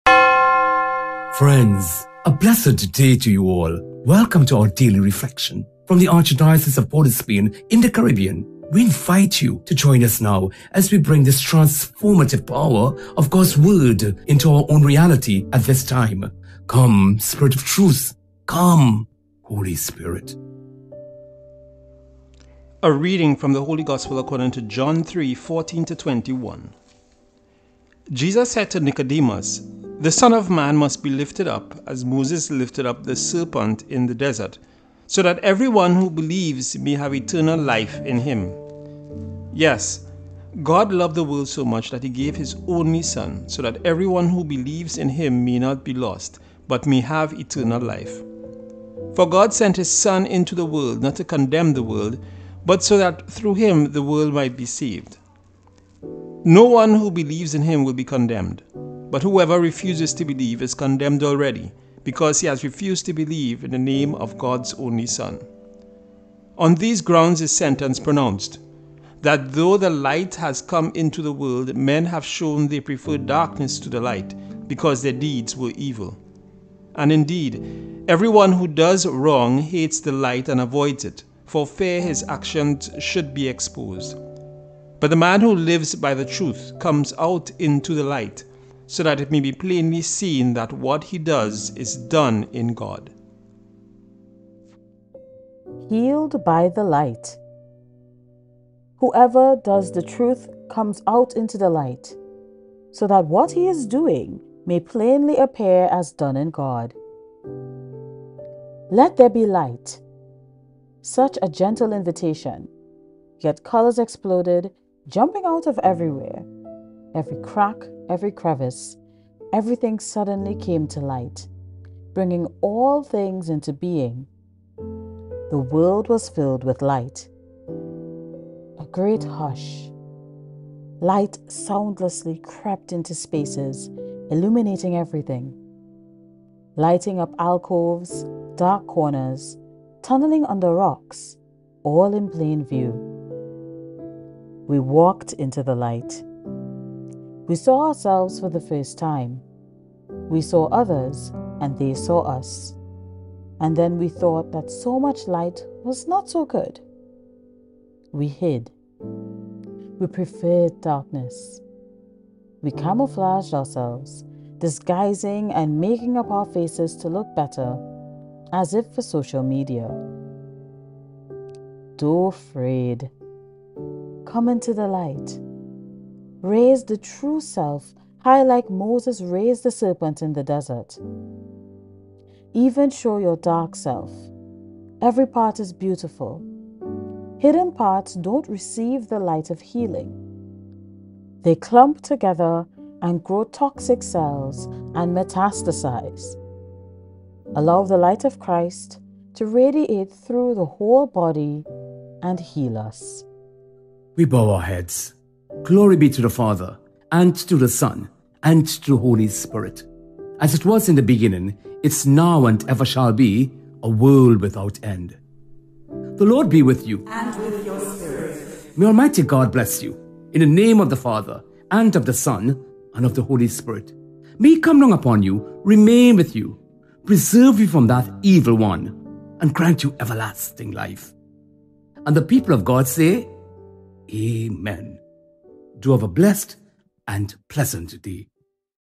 Daily Reflection 🇹🇹🇹🇹: Sunday March 10, 2024 - Week 4 in Lent (B)
Join us daily from CatholicTT as we bring you local faith formation for Lent from Trinidad and Tobago 🇹🇹🇹🇹